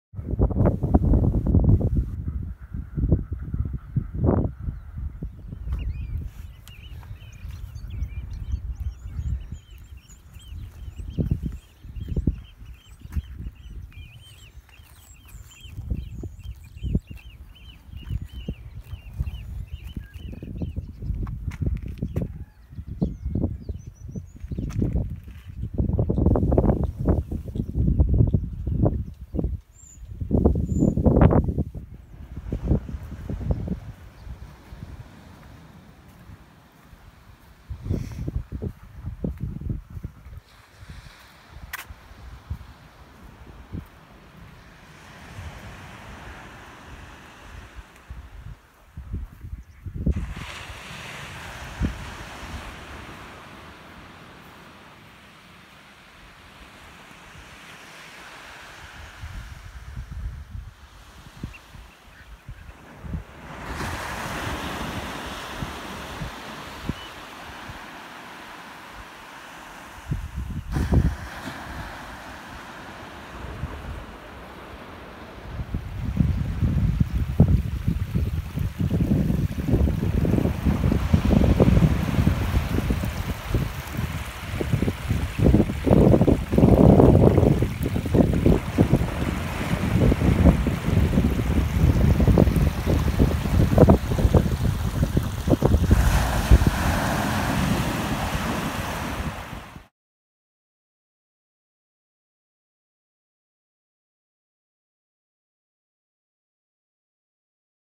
Shetland Adventures - 2 - Maywick
Here’s the second instalment of my Shetland Adventures series. This time, I’ve been in Maywick, also near Bigton in the south mainland.
Shockingly, I’d never actually been here before, and it was a strong recommendation from my friend Rebecca that finally tipped me over the edge, and I wasn’t disappointed!